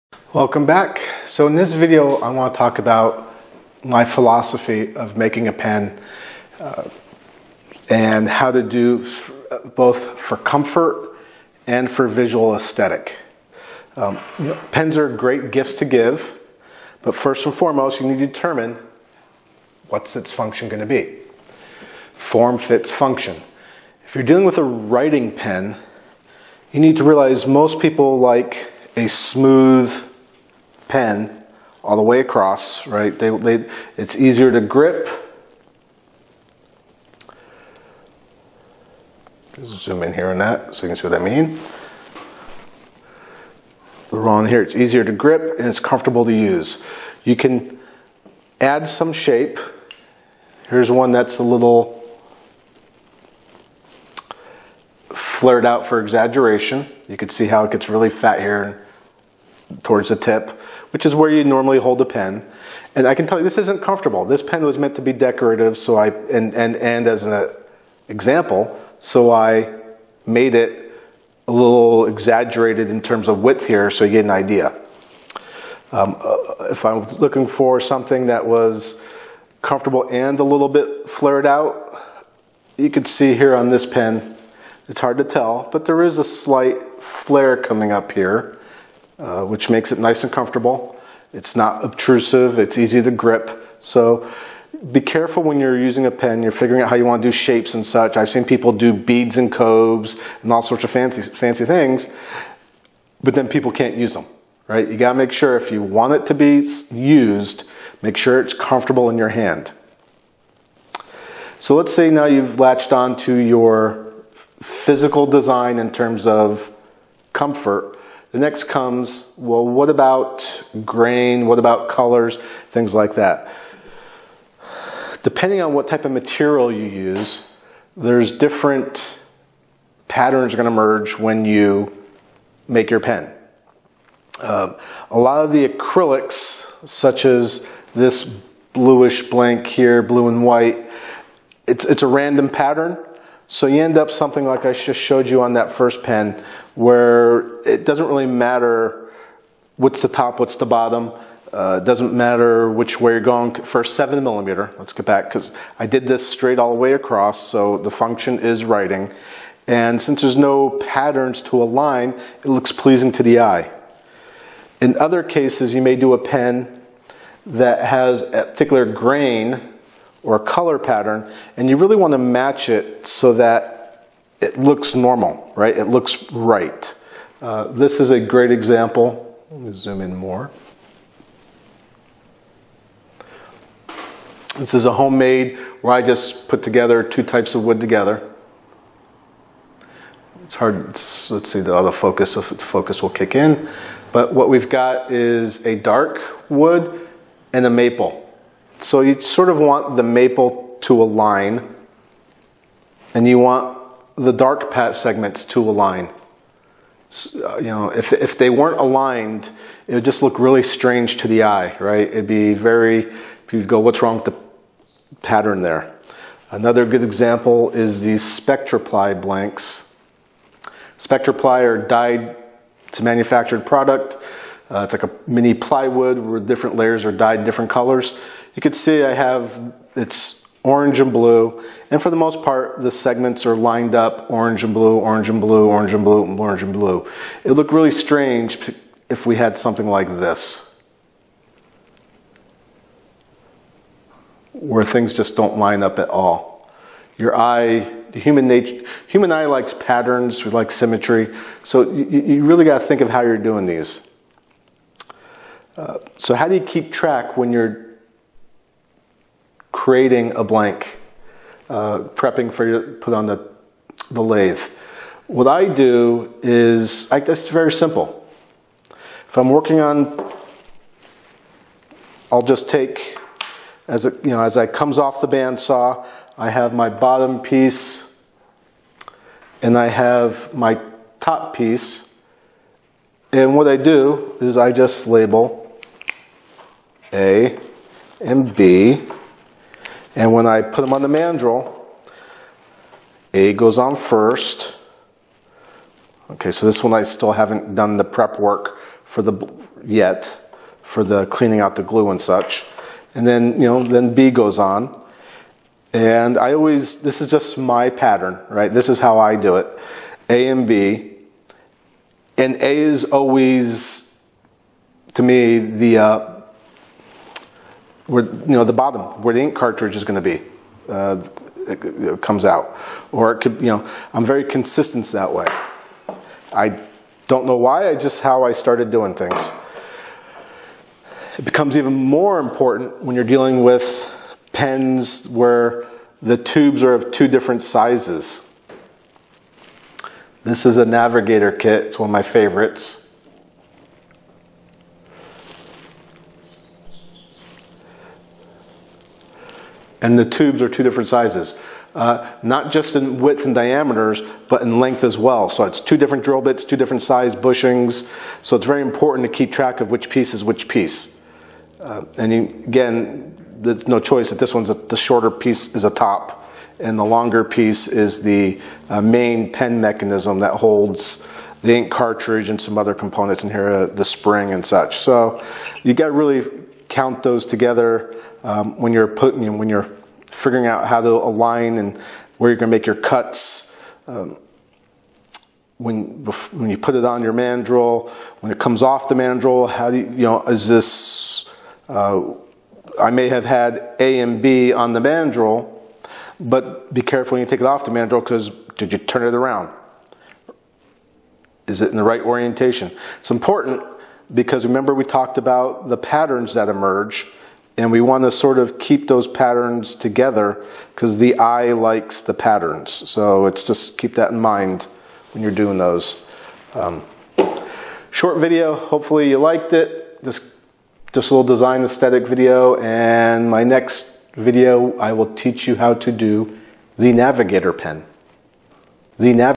EP 04 Pen Design Philosophy
In this video, I discuss my philosophy on pen design: shape, grain patterns, and more.